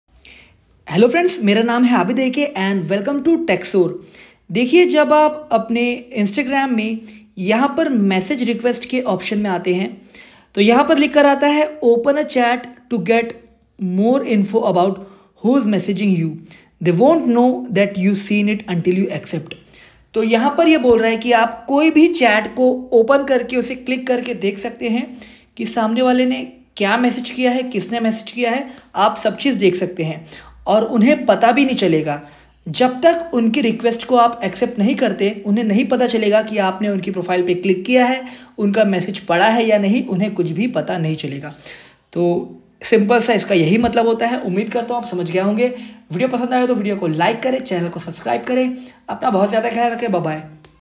Instagram | Open a chat to get more info about who's messaging you
They won't know that you've seen it until you accept

Talk soon,
Abid Akay